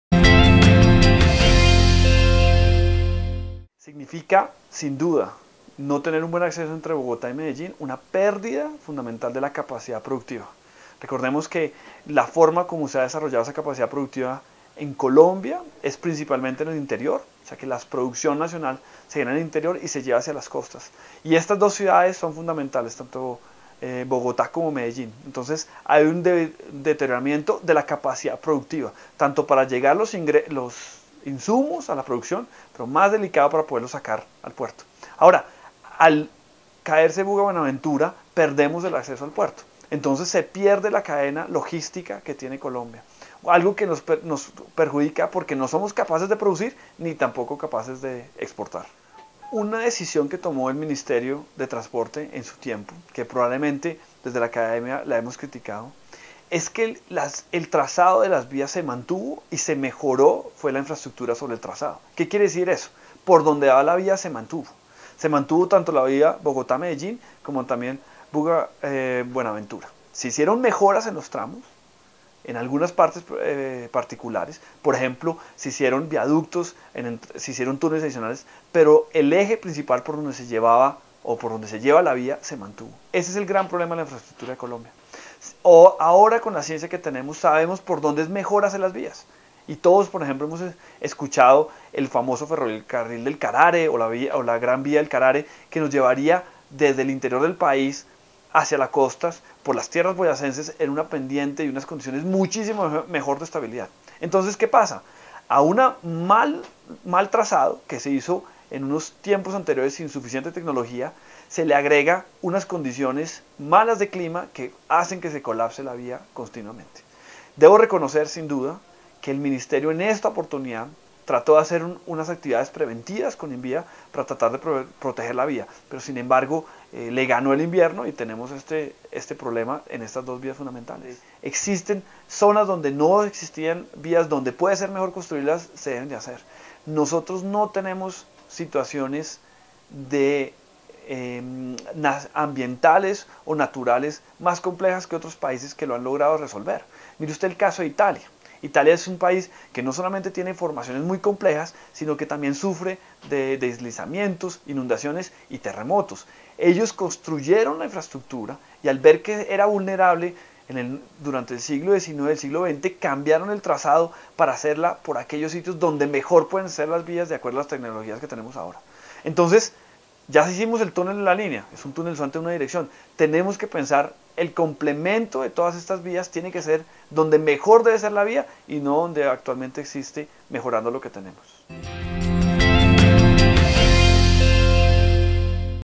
Derrumbes en las principales vías del país
Daniel Páez explica qué significa para el país que estas importantes vías tengan problemas ocasionados por el clima.